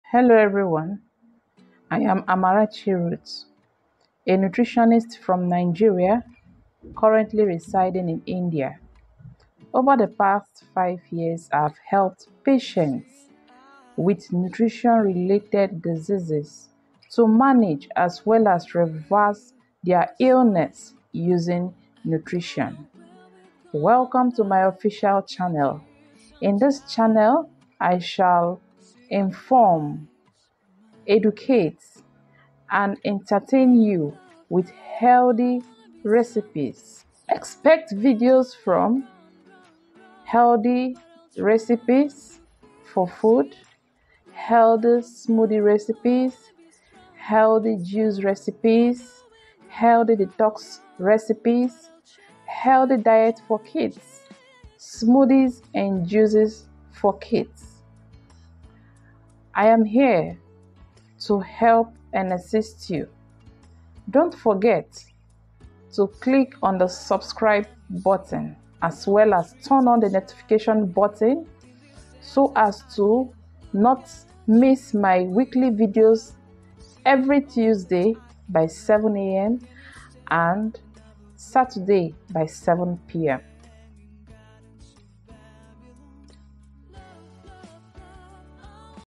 Welcome to My Channel
Welcome to the official YouTube channel of YOU N' YOUR HEALTH.

On this channel I will be making videos to inform, entertain and educate  you about everything on healthy diet and healthy living so as to help you make the right choice.

Expect Videos on

Healthy weight loss diet
Healthy diet recipes
Healthy smoothies recipes
Healthy kids diet
Healthy smoothies and juices for kids